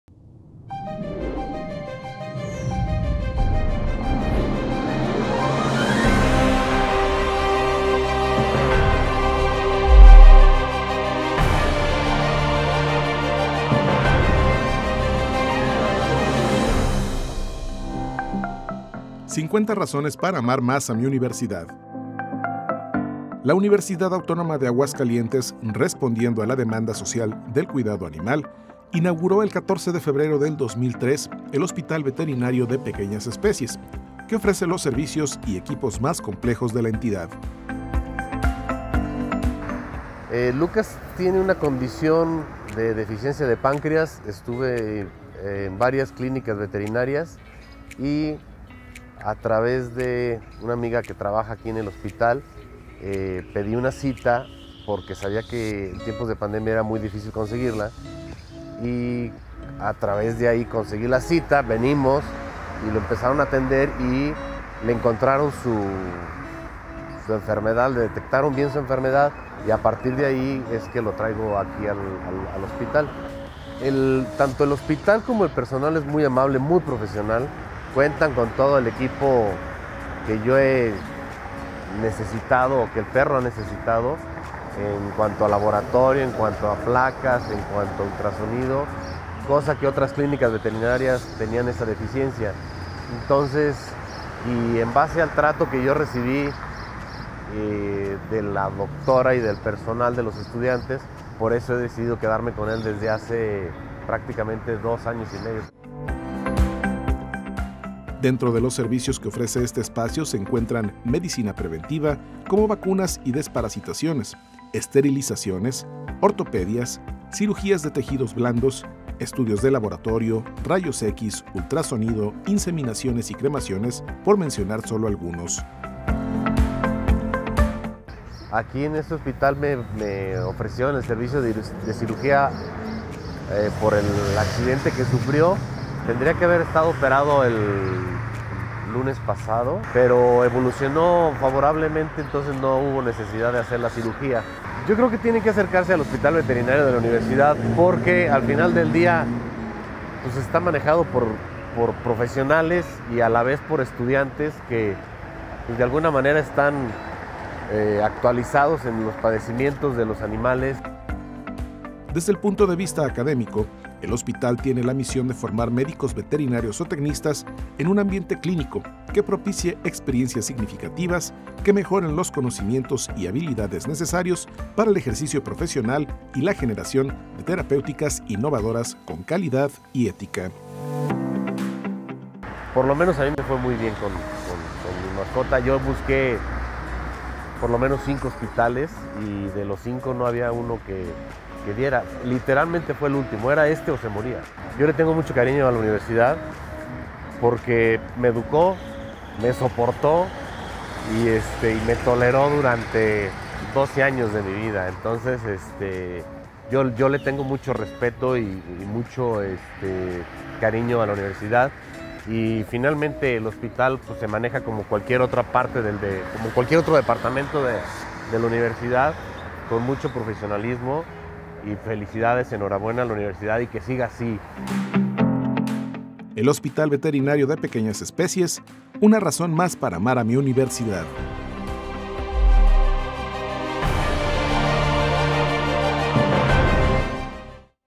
50 Razones para amar más mi Universidad - Hospital Veterinario
Una producción de UAATV con el motivo del 50 Aniversario de la Universidad Autónoma de Aguascalientes.